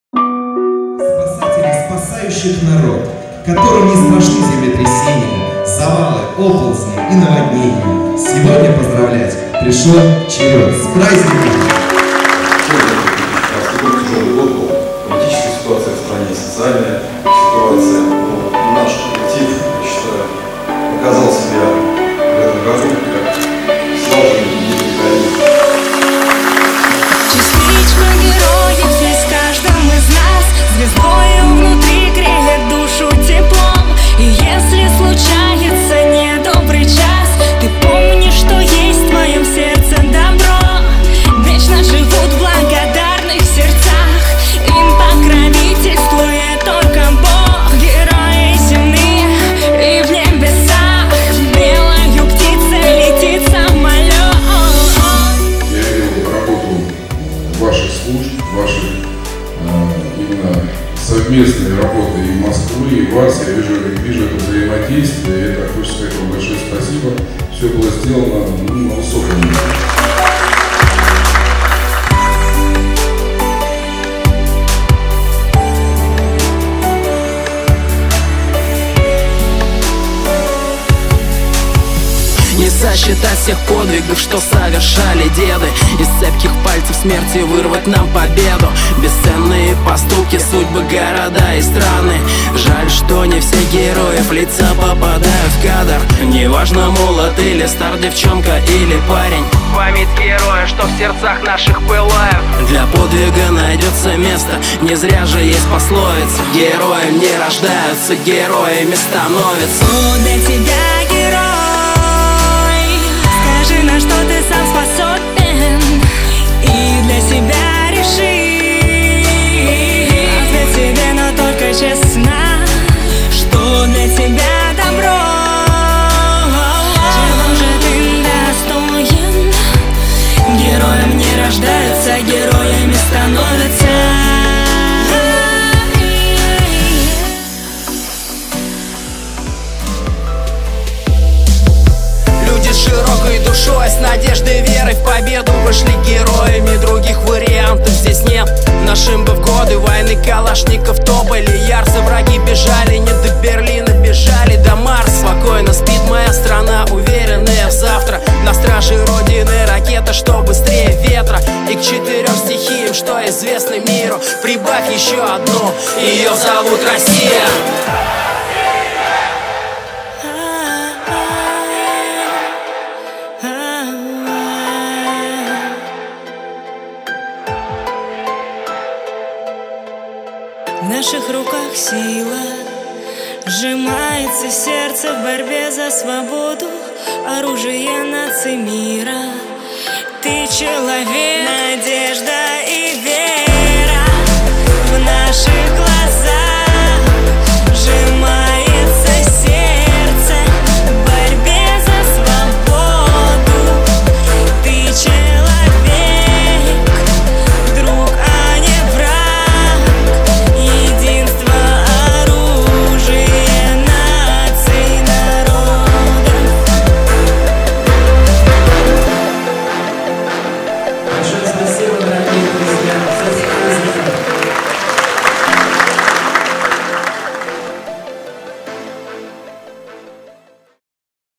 Группа "ЯРОПОЛК". День МЧС России, г. Красногорск, 25.12.2015 г.
Из архива СПК "ЯРОПОЛК".
25 декабря 2015 года в администрации Красногорского района прошло торжественно совещание, посвящённое юбилею "25 лет МЧС России".
Группа "ЯРОПОЛК" исполнила 2 песни перед 350 сотрудниками МЧС.⠀